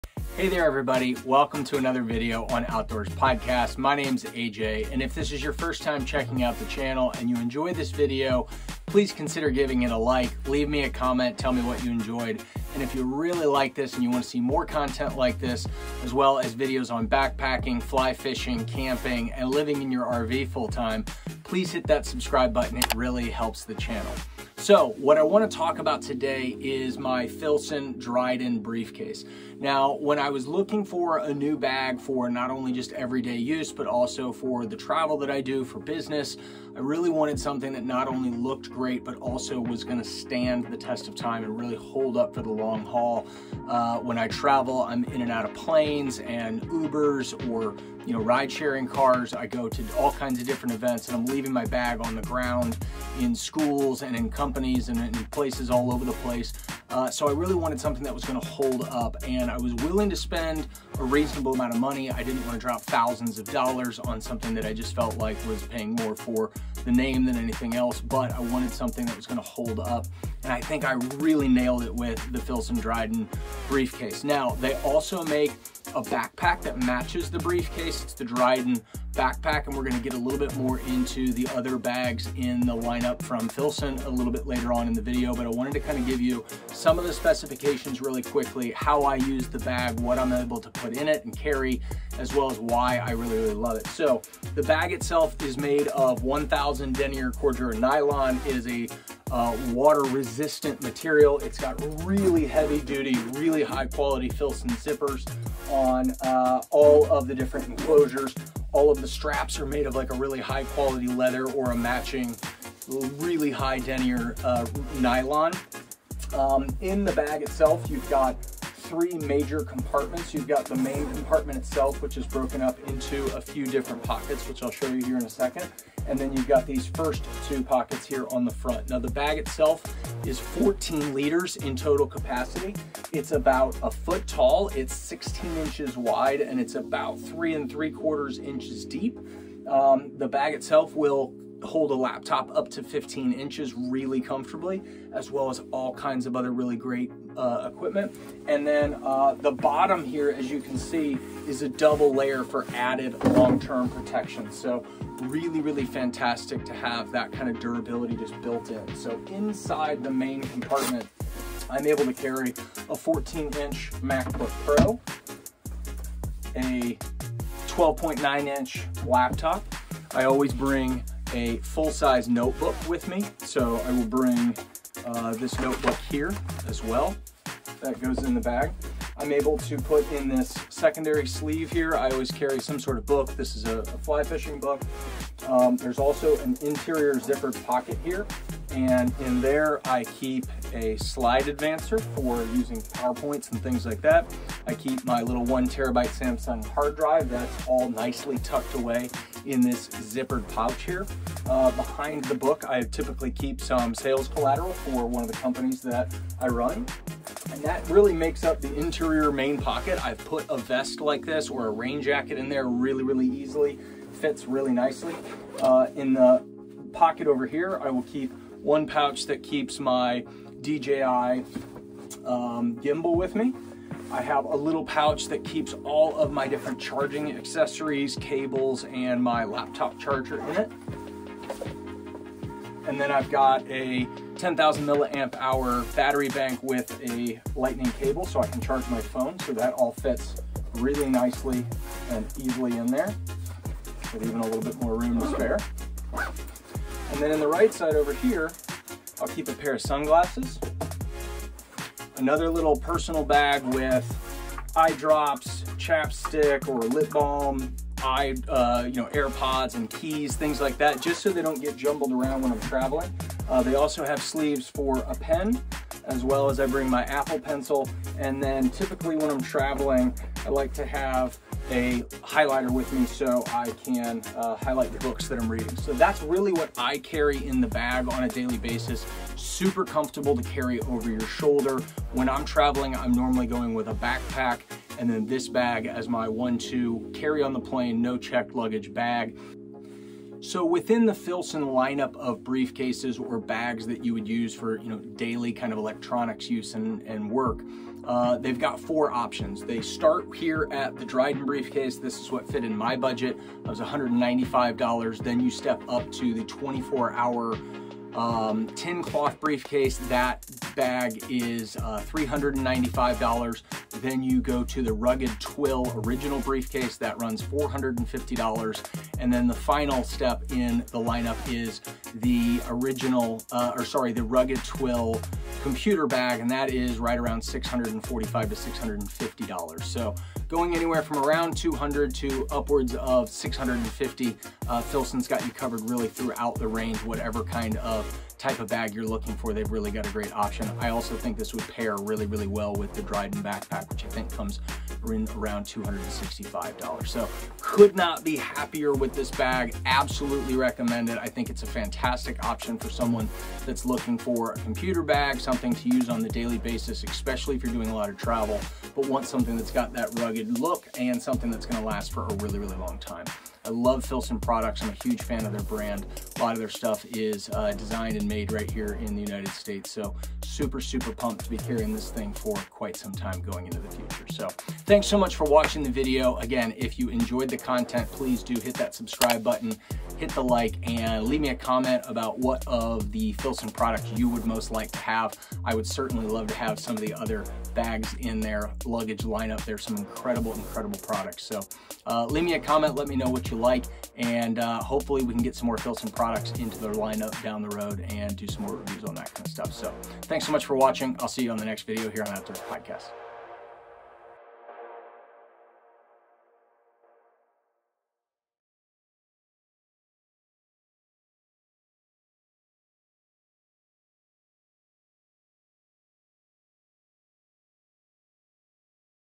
Filson Dryden Briefcase
The Filson Dryden Briefcase has been my go-to work bag for the last six months.  I was looking for a rugged briefcase that would hold up under heavy travel, but still look great in just about any scenario.

The bag is 14 liters in total capacity and measures 16" wide, by 12" tall and 3.75" deep from front to back.  Filson uses 1000D Cordura Nylon for the main body of the bag with a double layer on the lower section for added durability.  The interior is broken up into thoughtful pockets that make staying organized easy without having to search in 1,000 interior pockets for that charging cable you are looking for.

The Dryden falls at the entry point for Filson briefcases, providing three higher level options ranging from $195 to $625 dollars.

I've absolutely loved the Dryden Briefcase so far and hope to pair it with the Dryden backpack sometime down the road.

Podcast: Apple, Google, Spotify, iHeart Radio